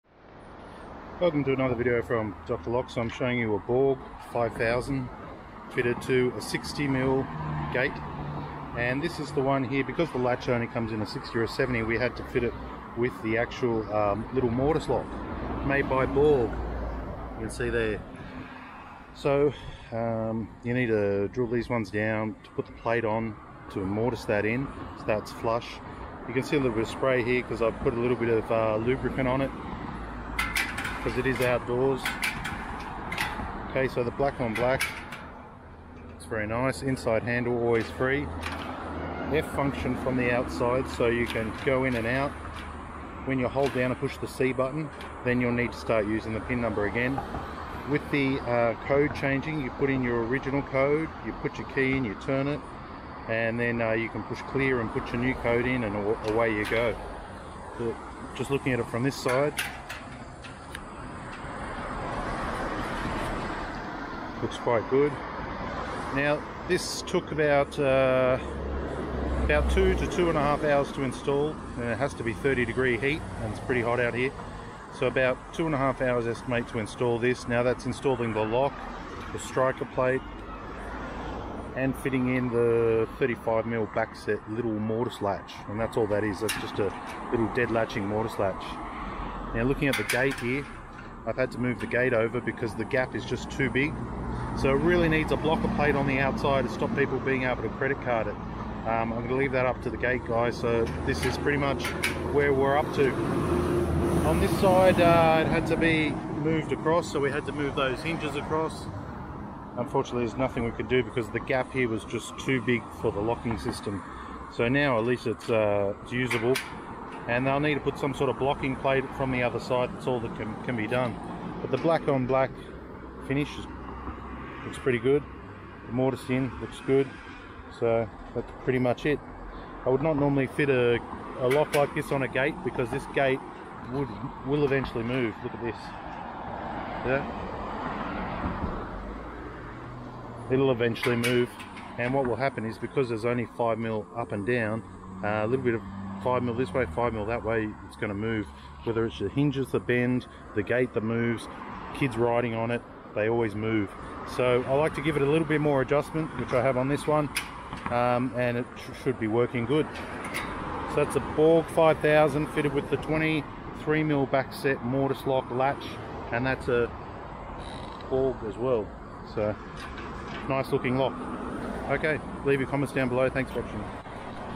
Digital Lock Black Borg 5000 With Borg Latch On 60mm Steel Gate
This is just a quick video to see a Borg 5000 digital code lock that has been fitted to a steel 60mm post gate.